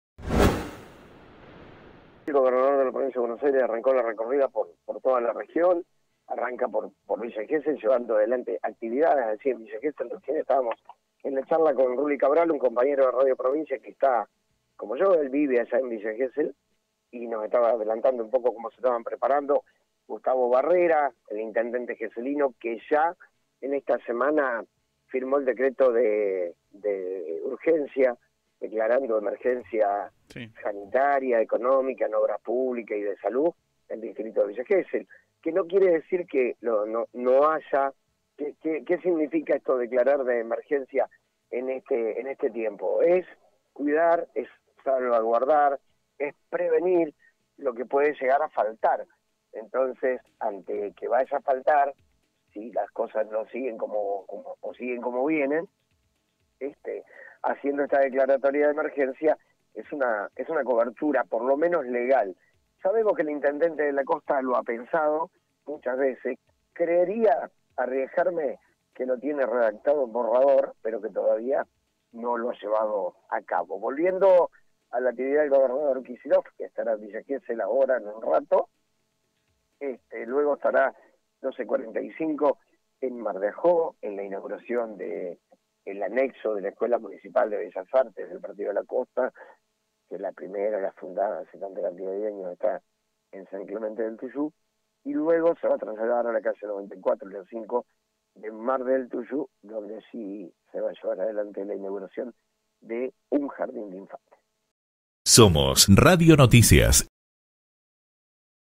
El gobernador Axel Kicillof visitará hoy La Costa e inaugurará dos escuelas
Comunicación con Quique Silva